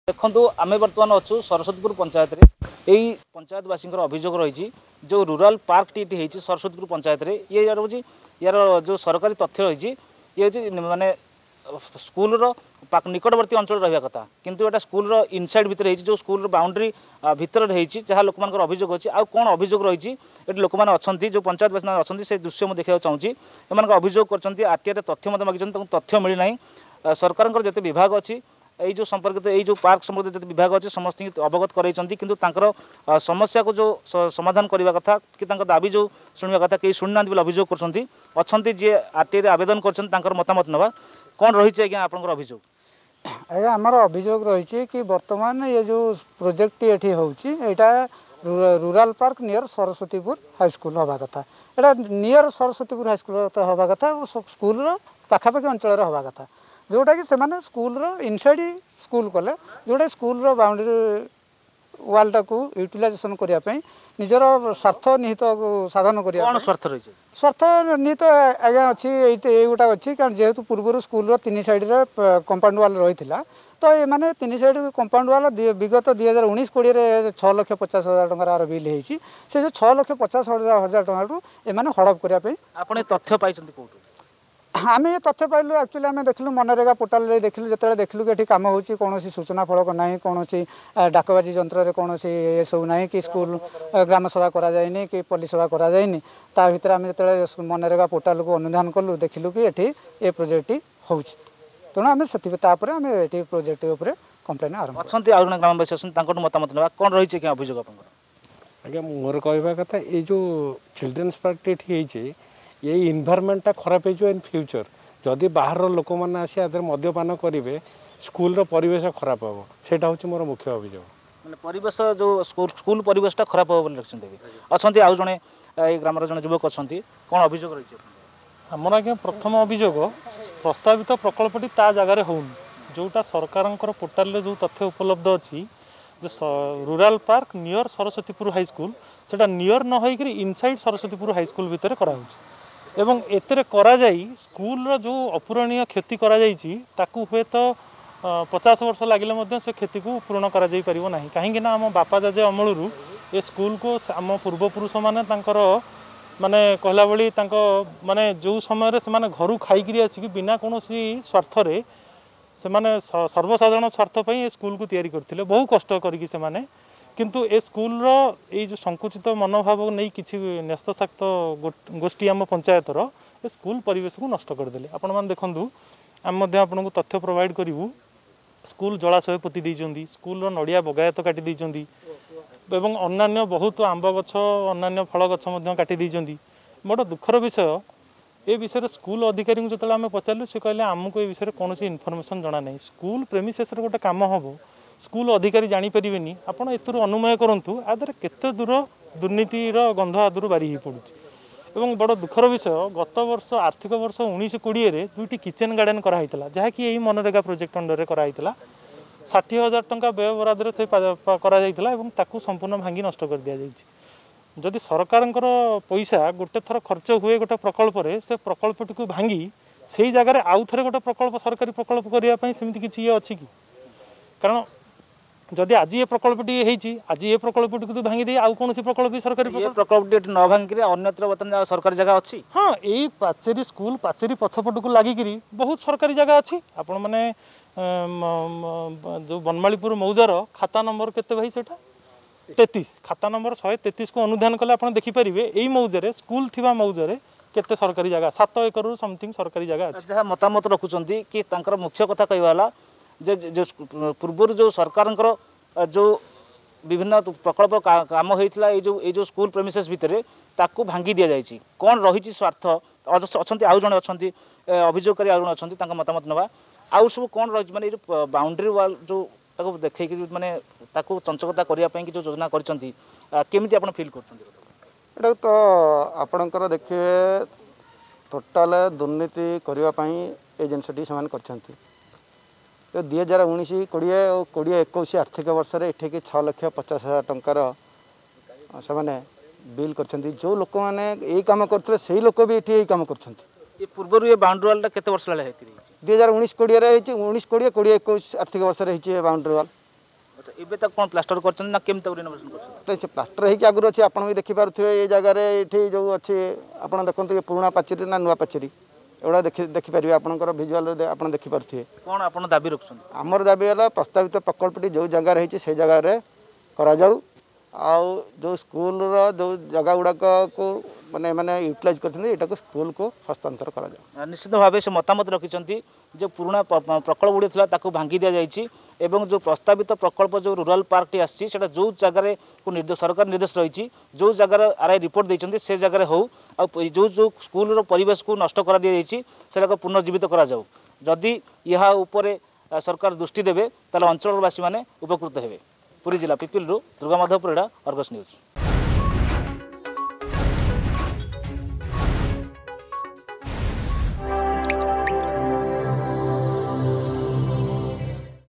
ପାର୍କ ନର୍ମାଣରେ ଦୁର୍ନୀତି ର ଗନ୍ଧ
ପିପିଲିର ସରସ୍ବତୀପୁର ପଞ୍ଚାୟତରେ ପାର୍କ ନର୍ମାଣରେ ଦୁର୍ନୀତି ଗନ୍ଧ ବାରିଲେ ସ୍ଥାନୀୟ ଗ୍ରାମବାସୀ । ଏହାକୁ ନେଇ ପ୍ରତିକ୍ରିୟା ରଖିଲେ ସରସ୍ବତୀପୁରର ସ୍ଥାନୀୟ ବାସିନ୍ଦା